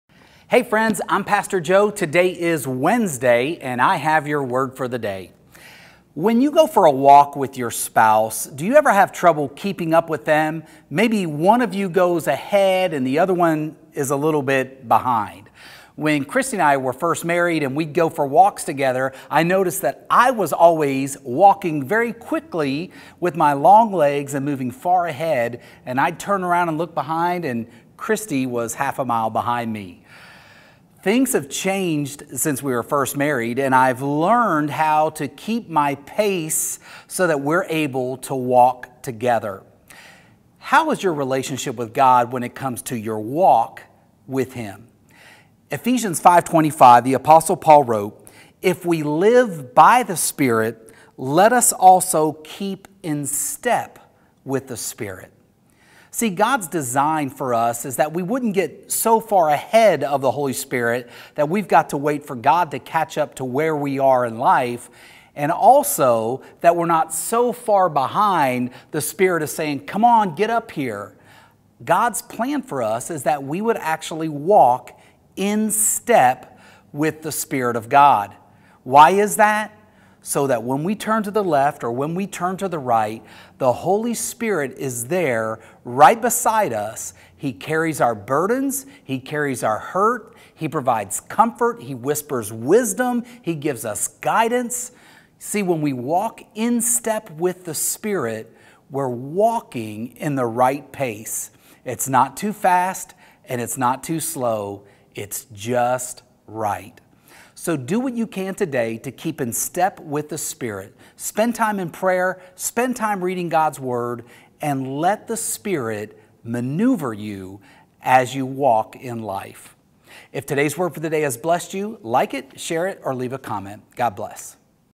Your Word for the Day - Episode 687
Pastor Joe encourages us to walk in step with the spirit of God.